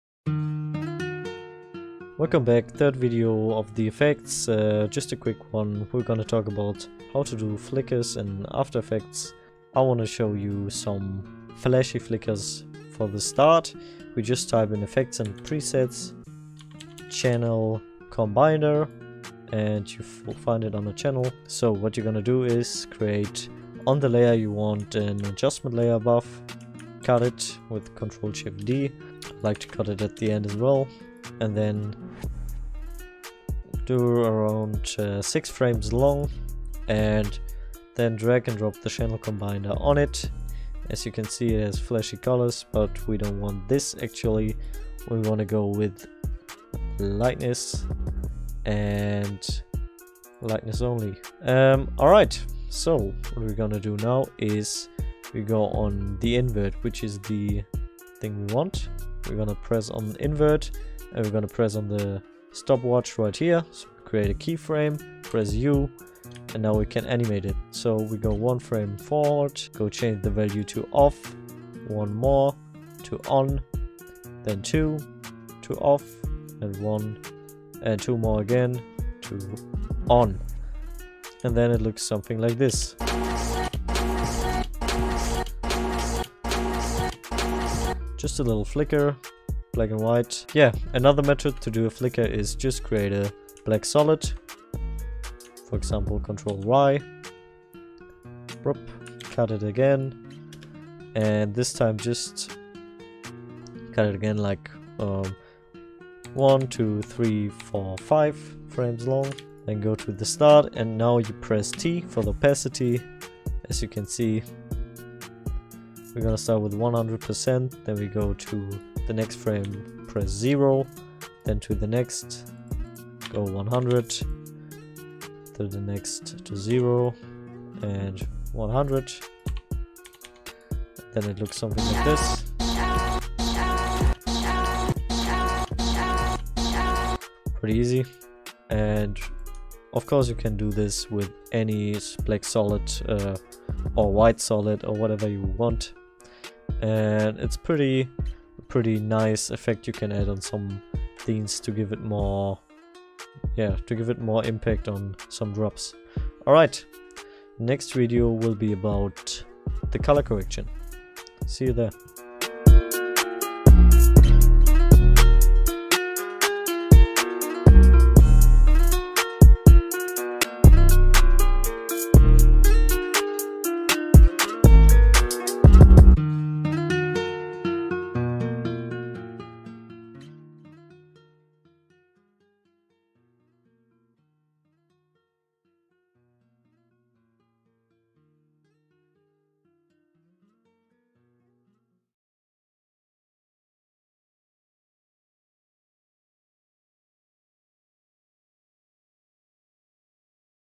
How to Do Simple Flicker in Anime Edits | After Effects Beginner AMV Tutorial #6.3
Part 6.3 of the complete AMV Tutorial series. In this video I will tell you how I do simple flicker.
Forgive me my non fluent english skills ^^ and enjoy! Part 6.3 will follow soon
0:00  INTRO
0:15  INVERT FLASHES
1:30  OPACITY FLICKER
2:30  OUTRO